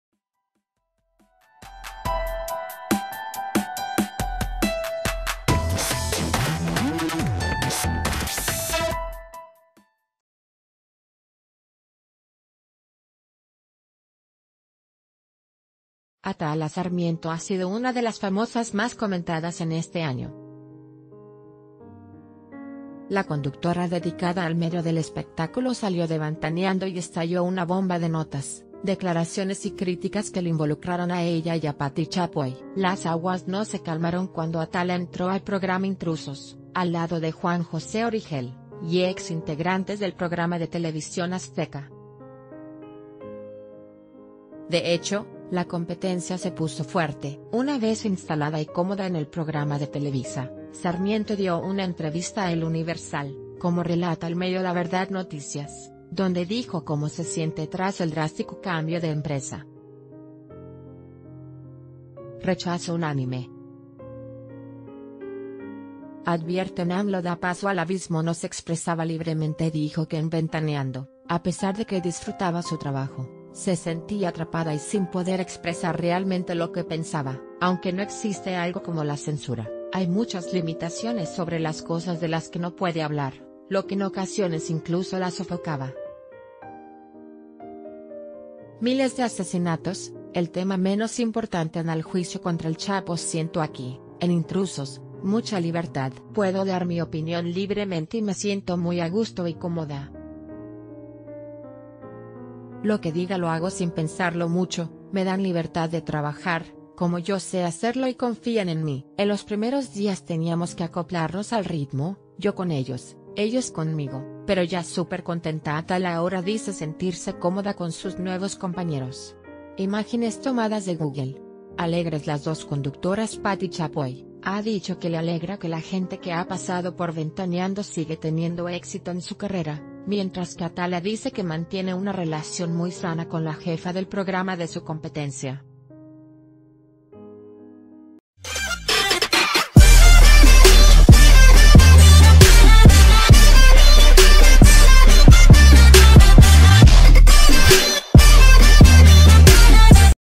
Atala Sarmiento confiesa un 'secreto' sobre trabajar en Ventaneando
Atala Sarmiento ha sido una de las famosas más comentadas en este año. La conductora dedicada al medio del espectáculo salió de Vantaneando y 'estalló' una bomba de notas, declaraciones y críticas que la involucraron a ella y a Pati Chapoy. 'Las aguas no se calmaron' cuando Atala entró al programa I...